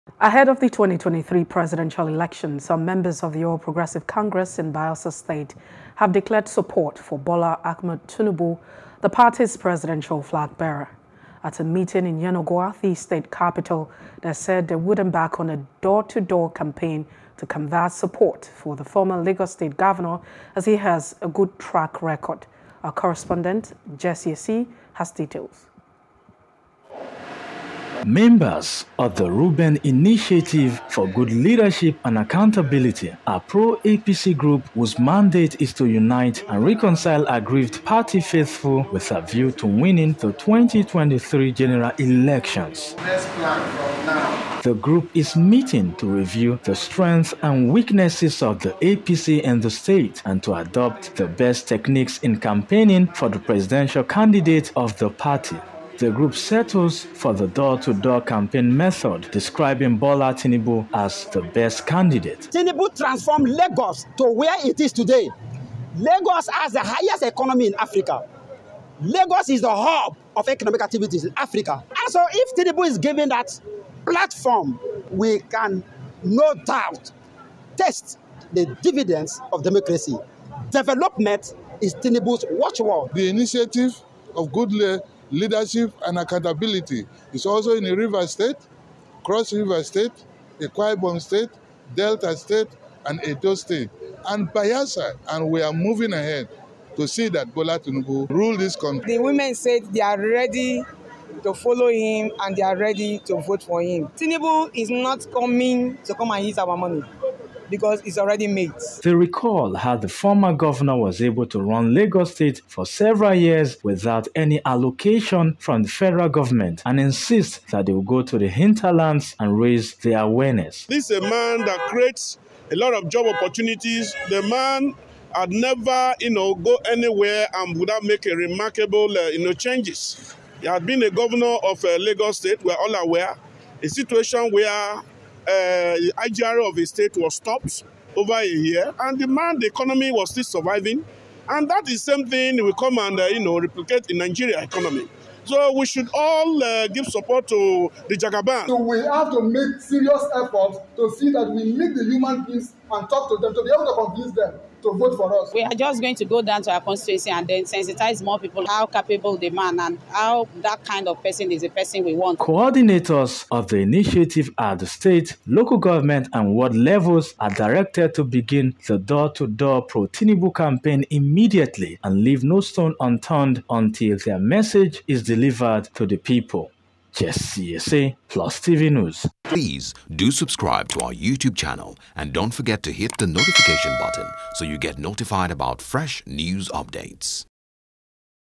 2023 Presidential Election: Pro-APC Group In Bayelsa Declares Support For Tinubu | NEWS
Ahead of the 2023 presidential election, some members of the All Progressives Congress in Bayelsa State have declared support for Bola Ahmed Tinubu, the party's presidential flag bearer.